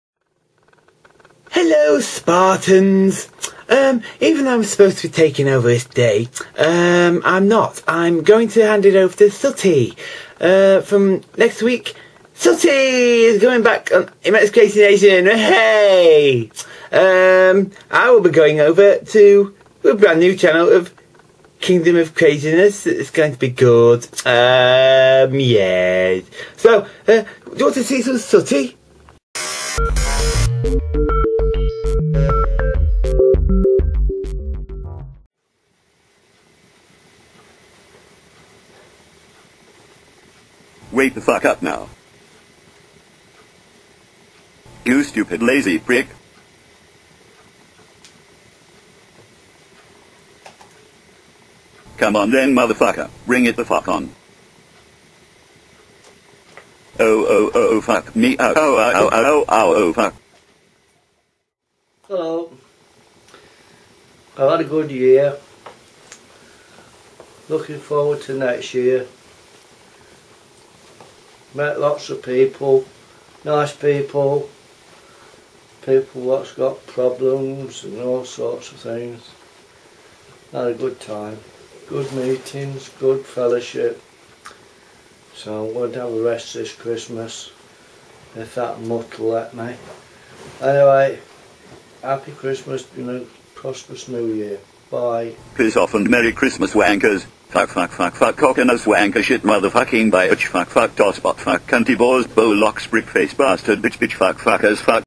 Tourettes Bear - Spartan - Week 27
This is Spartan's final video.  On Thursday 31st December when the change happens, Sooty will be back full time.  This includes a Christmes message from The Man.